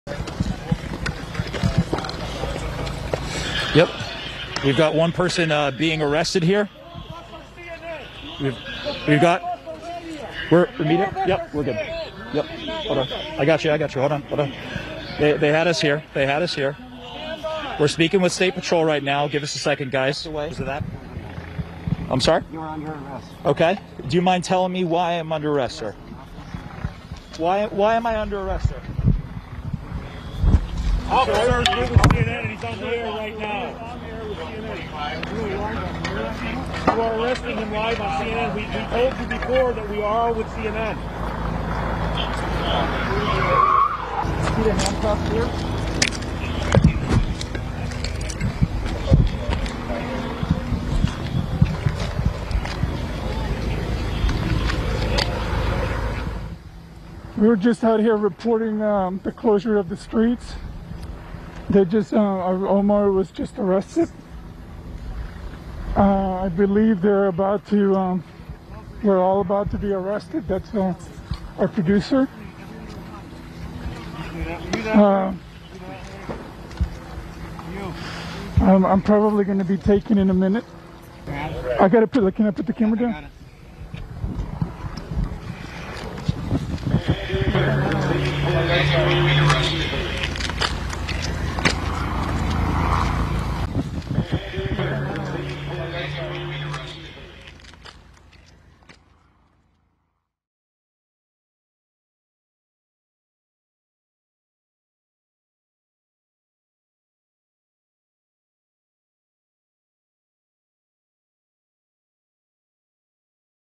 La policía de Minneapolis arresta en directo a un equipo de CNN que cubría los disturbios
El personal de la cadena estadounidense fue puesto en libertad pocas horas después de su detención injustificada.